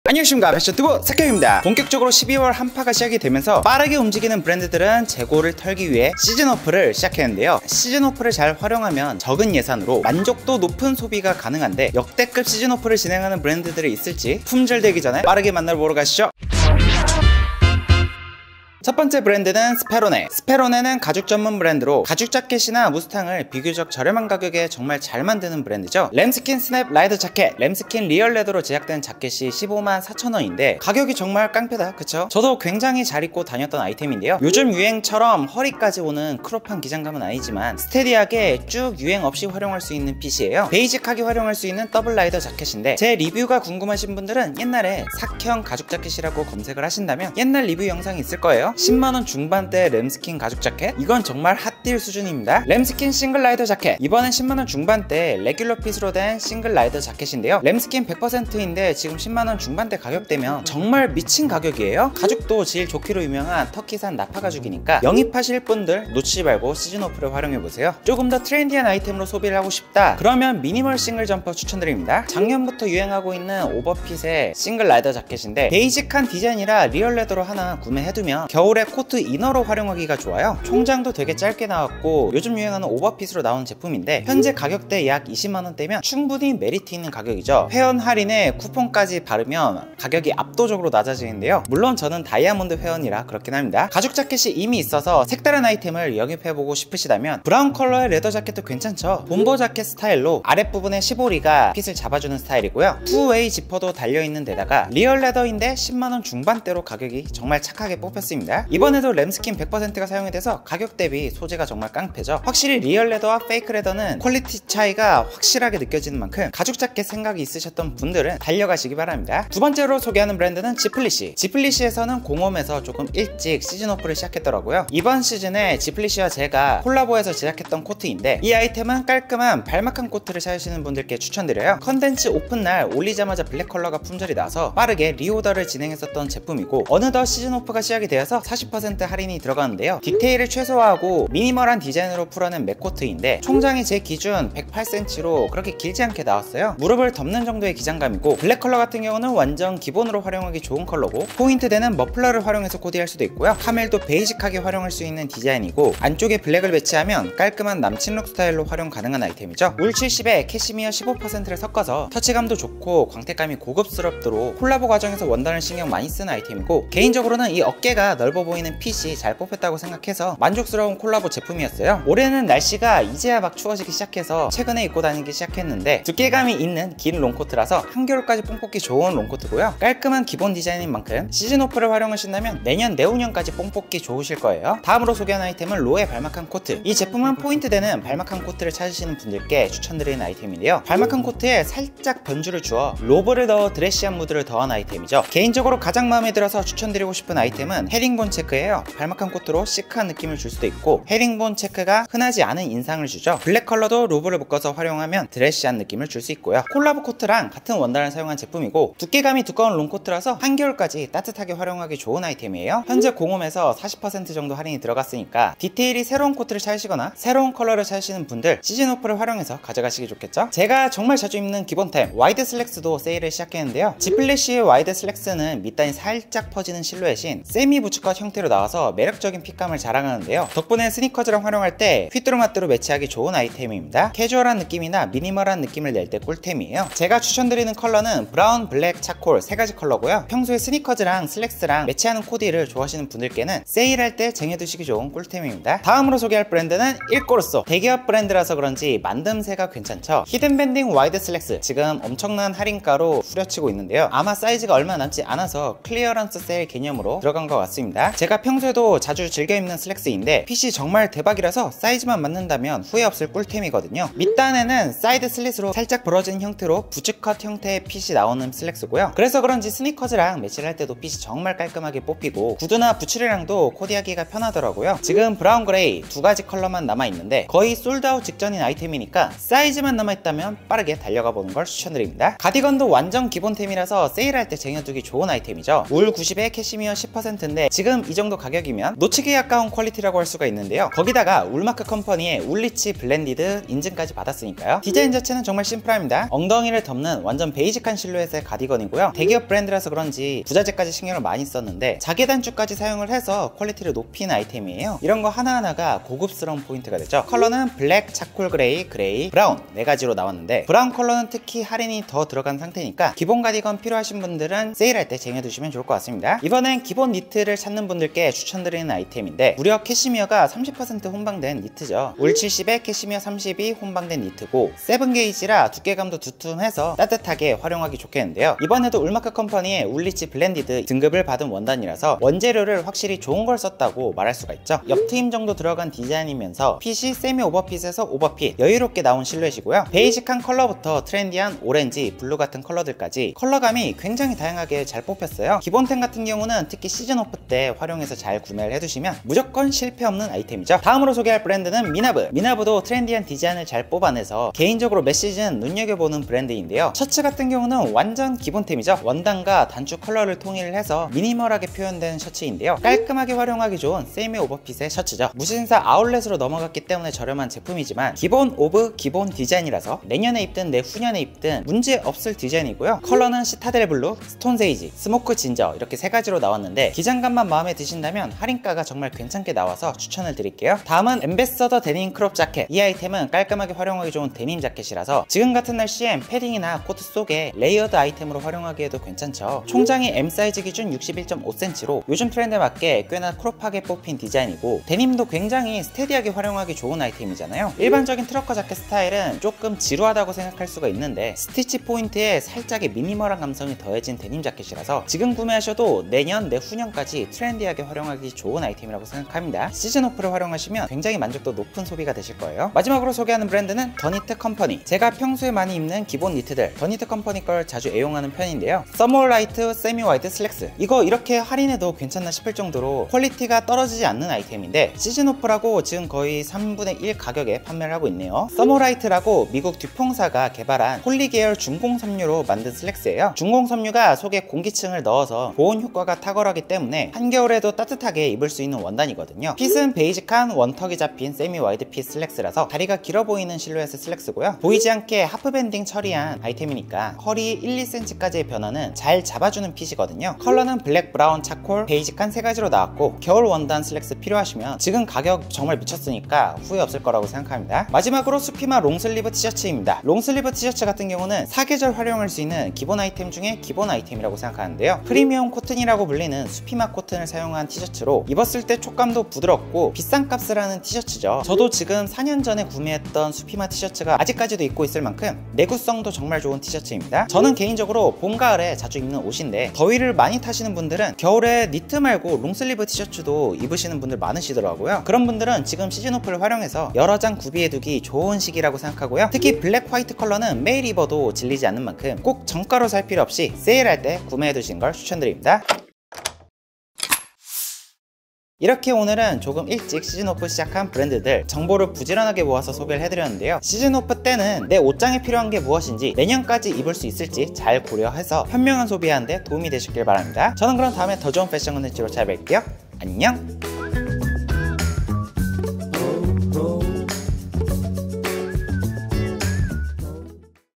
역대급 시즌오프 시작한 브랜드들! 품절되기 전에 함께 보시죠
오늘은 일찍 시즌오프 시작한 브랜드들 정보를 부지런하게 모아왔습니다! 추가로 피스워커도 블랙진 할인이 시작됐네요 몇 개 링크 남겨두겠습니다.

삭형 Spec.
166cm 60kg
상의 : 95
하의 : 28
신발 : 250

00:00 intro
00:18 스페로네
00:27 스냅 라이더 자켓
00:53 램스킨 싱글 라이더 자켓
01:08 램스킨 미니멀 싱글 점퍼 맨
01:34 램스킨 오벌 포켓 자켓
01:59 지플리시
02:05 CASHMERE MAC COAT
03:05 지플리시 LOE BALMACAN COAT
03:40 WIDE SLACKS
04:06 일꼬르소
04:10 히든밴딩 와이드 슬랙스
04:42 블랙 에센셜 세미오버핏 캐시미어 혼방 가디건
05:16 에센셜 세미오버핏 캐시미어 라운드 니트
05:51 미나브
05:57 터키쉬 코튼 텐셀 셔츠
06:21 엠버서더 데님 크롭 자켓
06:53 더니트컴퍼니
06:59 써모라이트 세미와이드 슬랙스
07:38 수피마 롱슬리브
08:17 마무리

edit by 고종찬
촬영장비 : 소니 A7S3 / A7M3 / A6400